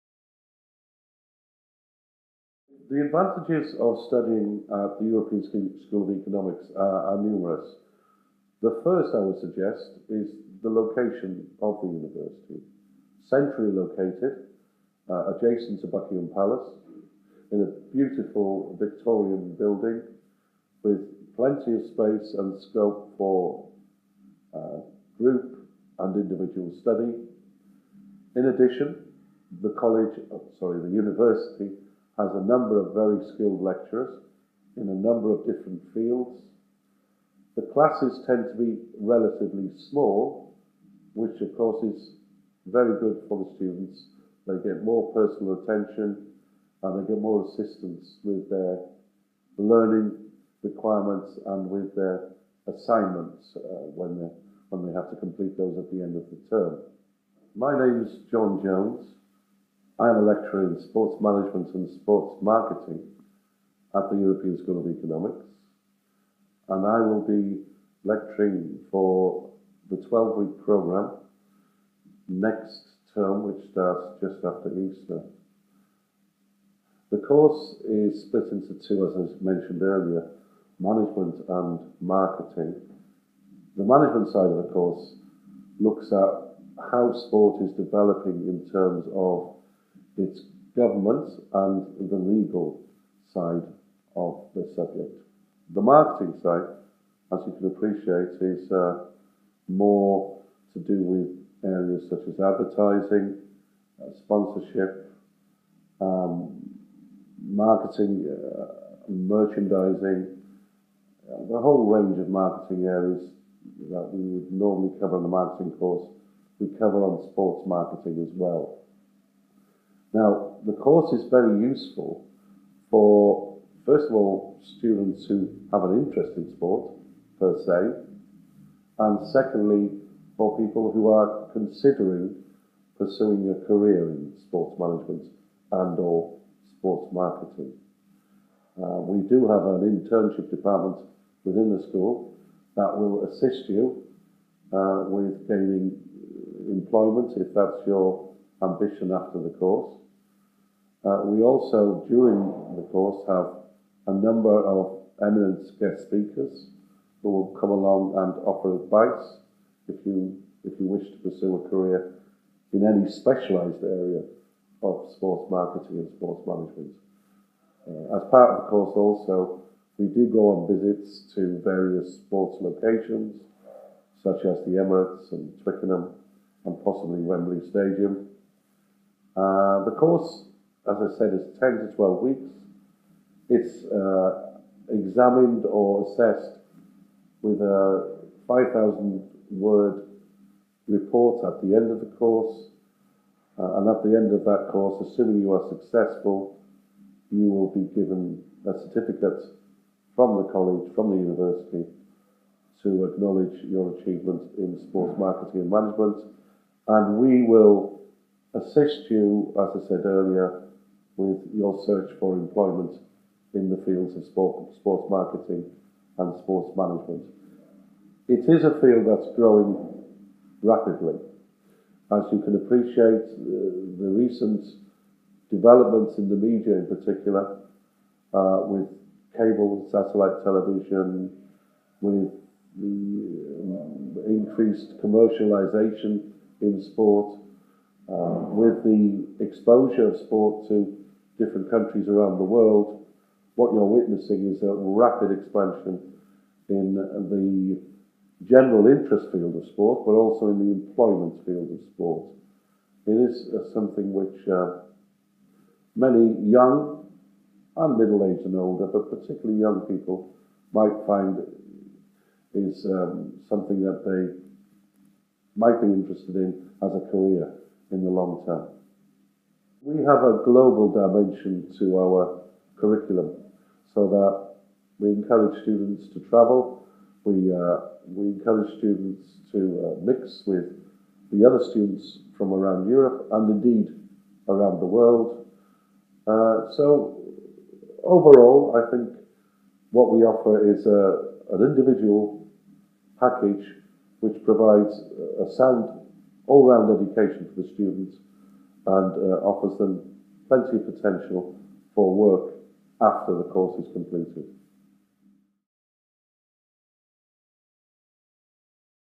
European School of Economics Lecturer, John Jones (Sports & Marketing)
Lecturer's course review at the European School of Economics London - Certificate Program in Sport Management and Sport Marketing.